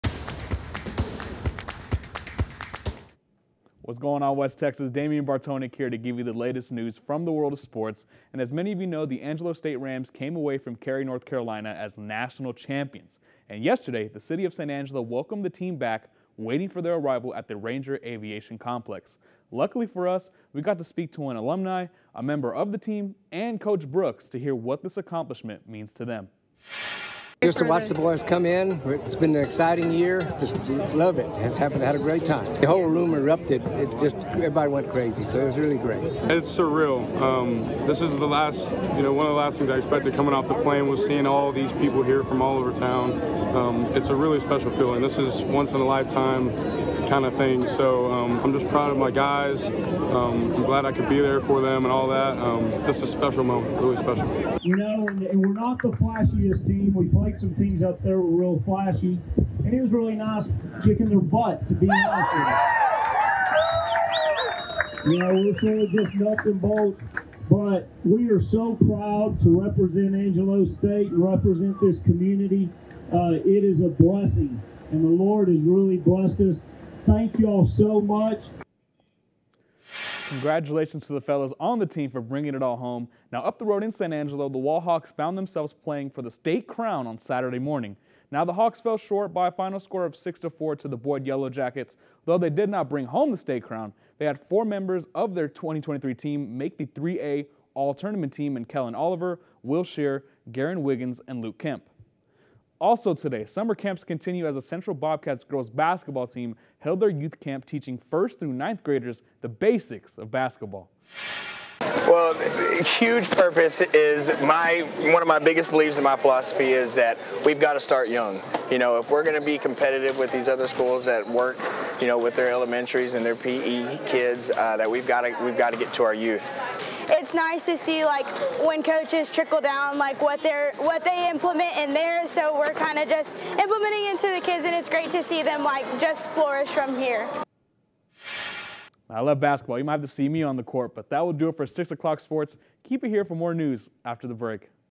The Angelo State Rams baseball team is welcomed home by the locals
The 2023 national champions arrived in style with plenty of love and support given to them on Sunday afternoon.